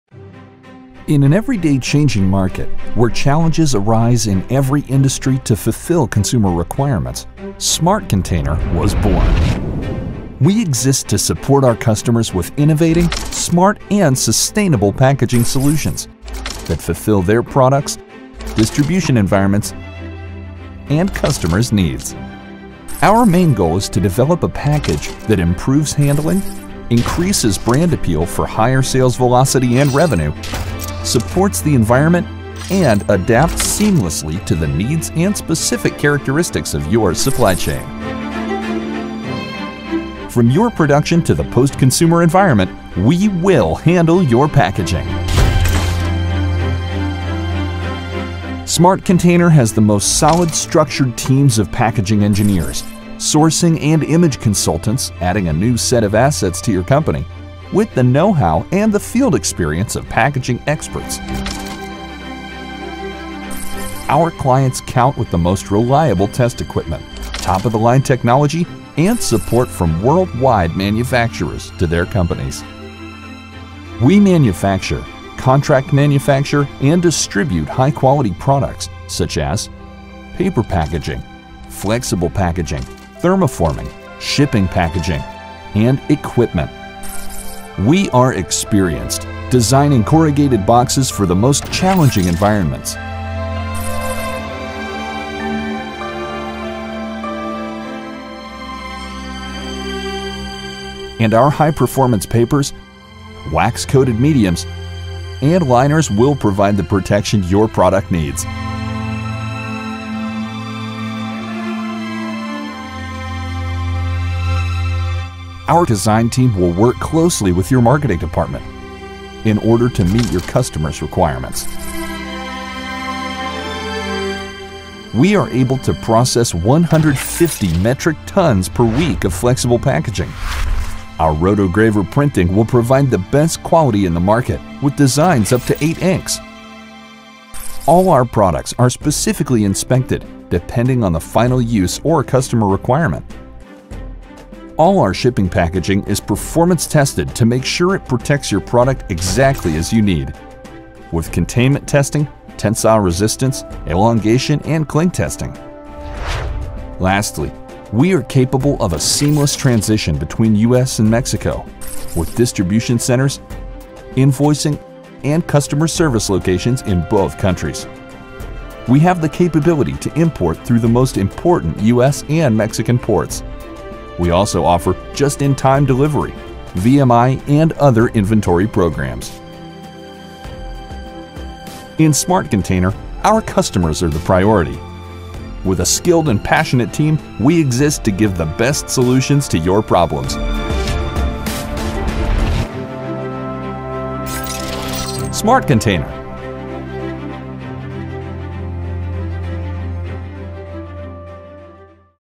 SmartContainer - Corporate Video Narration
A corporate narration I did for SmartContainer.